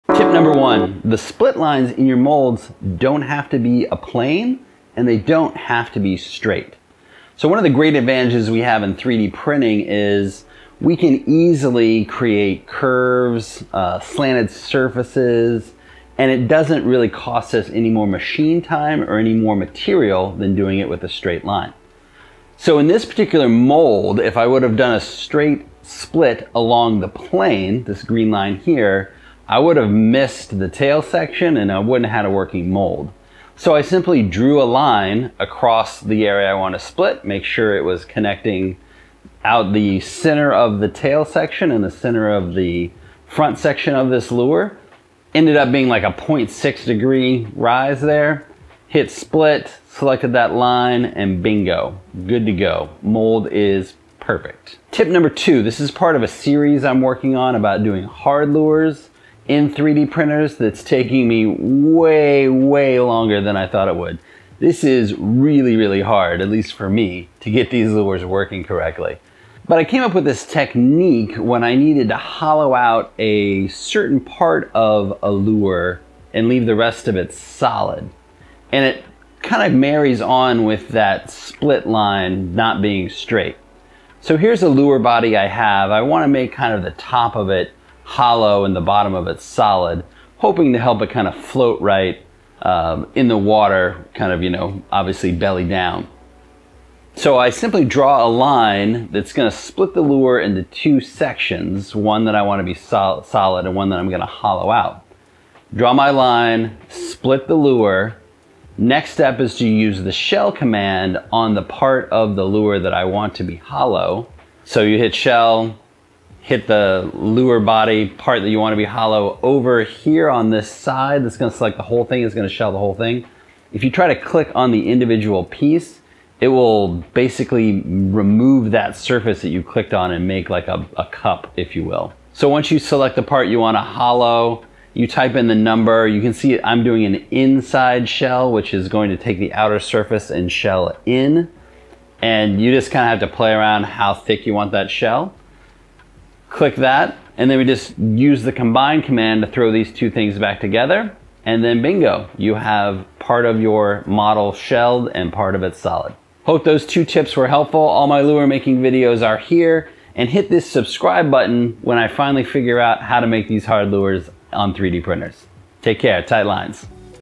2 Quick and dirty 3D Printed Lure Design Hacks
Can't wait to see the cool things you build with these hacks.

00:00 - Tip #1 You don't have to split straight
00:54 - Tip #2 Break it apart and put it back together